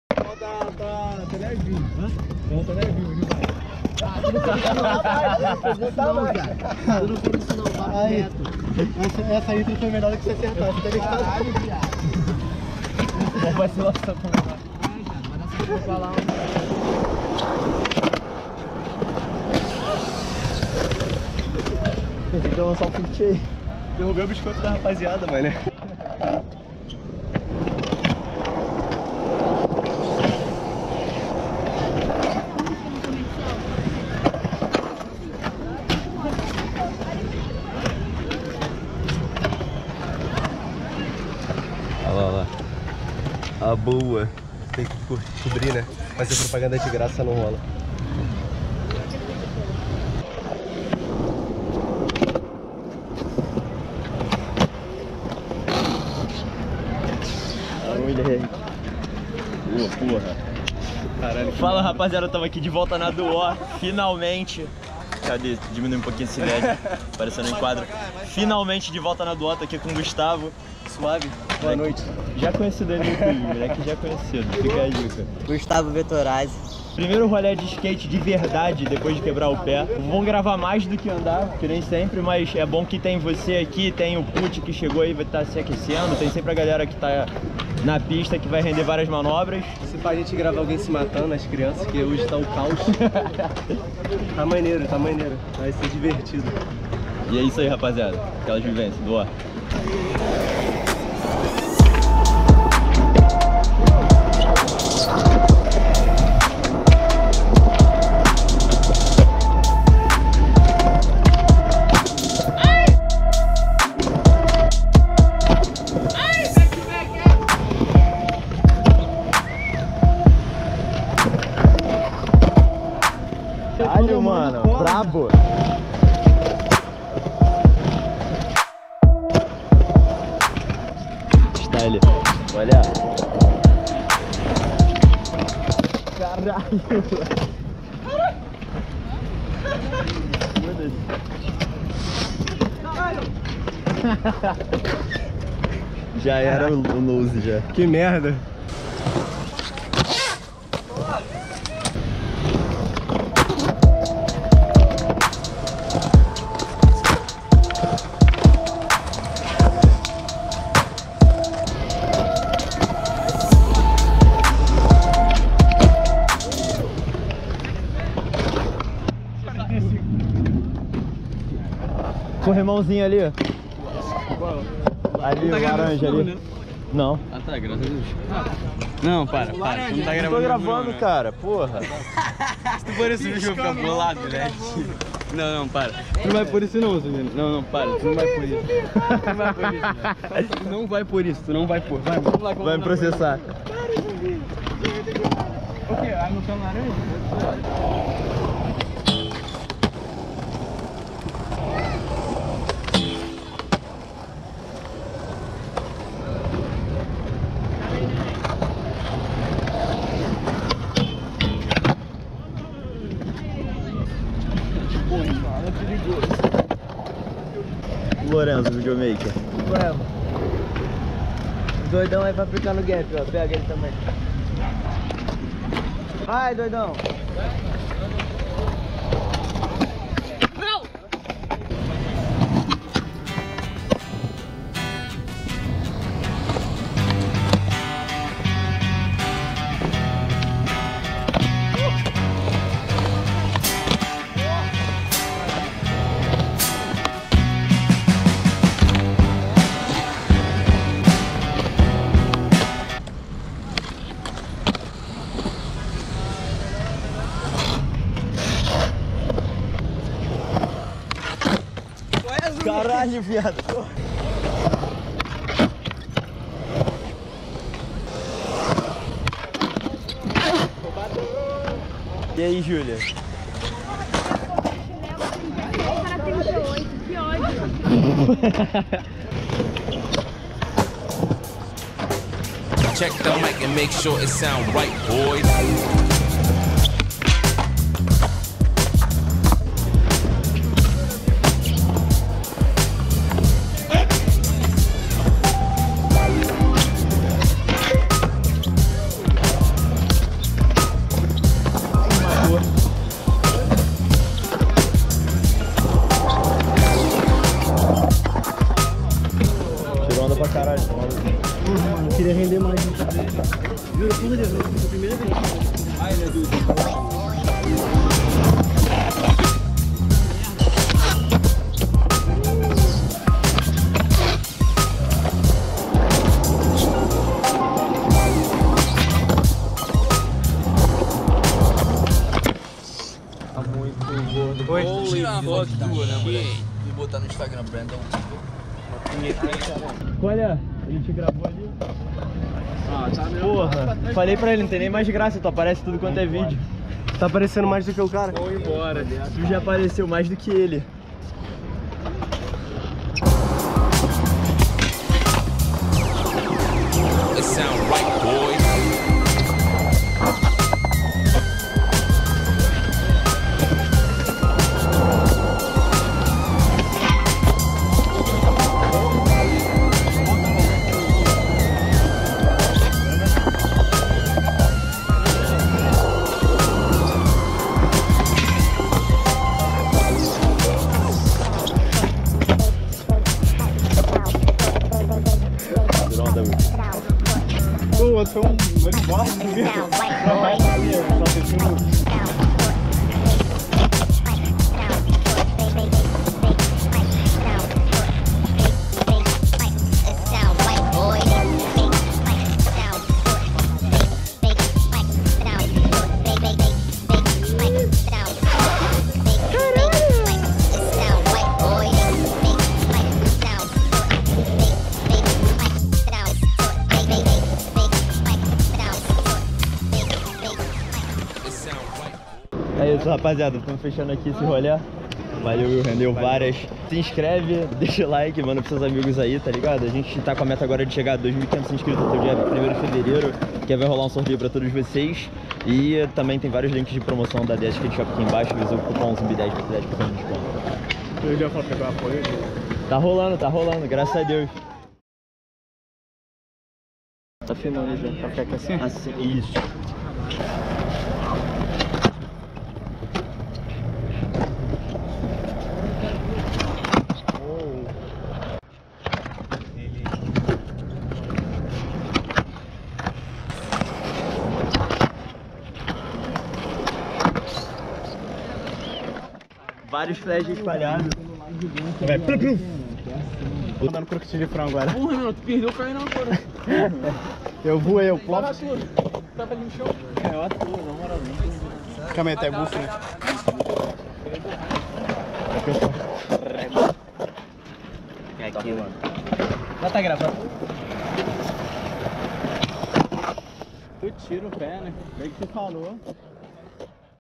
DE VOLTA NA DUÓ - ROLÉ DE SKATE COM OS CRIAS
Finalmente voltei pra gravar na Duó depois de tirar o gesso do pé e os crias representaram rendendo VÁRIAS.

Camisas a partir de R$ 49,90 (High, Champion, Hocks, Starter e etc)

Scorpion Handle (com o adaptador para GoPro):

Flash 96 Leds: